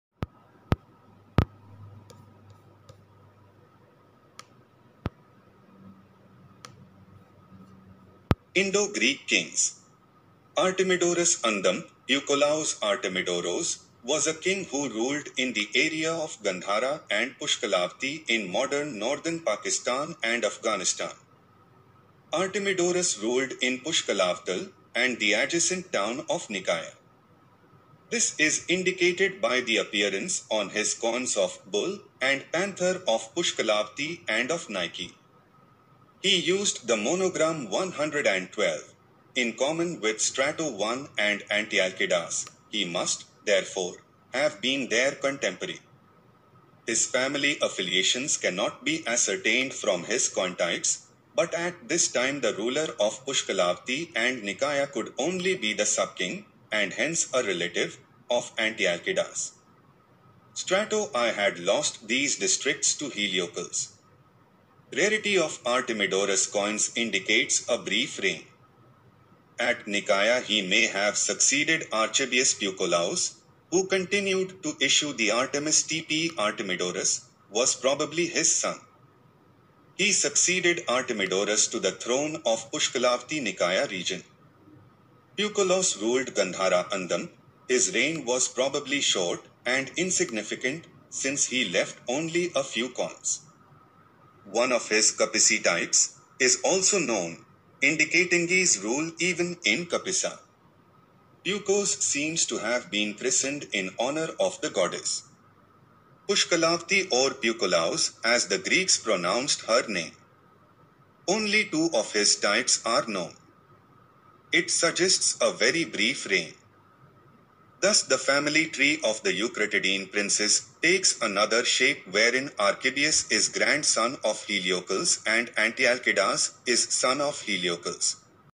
Ancient Indian Inscriptions Vol 1
Artemidoroswas a king who ruled in the area of Gandhara and Pushkalavati in modern northern Pakistan and Afghanistan.
Artemedorus ruled in Pushkalavatl and the adjacent town of £Nikaea. This is indicated by the appearance on his
coins of "bull and "panther' of Pushkalavati and of Nike.
He used the monogram 112, in common with Strato 1
and Antialkidas, He must, therefore, have been their contemporary. His family afiliations cannot be ascertained
from his coin-types, But at this time the ruler of Pushkalavati and Nikaea could only be the sub-king, and hence a
relative, of Antialkidas. Strato I had lost these districts to Heliokles. Rarity of Artemedorus coins indicates a brief reign. At Nikaea he may have succeeded
Peucolaos, who continued to issue the Artemis tpe Artemedorus, was probably his son. He succeeded Artemedorus to the throne of Pushkalavati Nikaea region.
Peucolaus (means Saviour) ruled Gandhara & his reign was probably short and insignificant, since he left only a few coins.
One of his Kapisi types, is also known, indicatinghis rule even in Kapisa. Peucoaos seems to have been
christened in honour of the goddess. Pushkalavati or Peucolaos as the Greeks pronounced her name. Only two of his types are known. It suggests a very brief reign.
Thus the family tree of the Eukratidean princes takes another shape wherein Archibius is grand son of Heliokles and Antialkidas is son of Heliokles.

References
M.V.D. Mohan Indo Greek Coins, Pages 132, 133 &
p. 196, Pl.VI 6.
(Pushkalavati Nagar devata type of an unknown king)
City goddess of Pushkalavatl
wearing a crown surmounted with
 Lotus flowers,
clad in sari, holding Lotus in right hand. Pakhalavadi davata
Broken Khroshthi legend, but restored as Kali (Amba) by
P L  Gupta JNSI XVII 1955 Pt.I Page 70q
(Artemidoros, whose name means "gift of Artemis," chose Artemis as the featured deity on his coins. Artemis, the goddess of the hunt, is recognized by her attributes, the bow and the quiver)
Reverse Indian humped bull Top.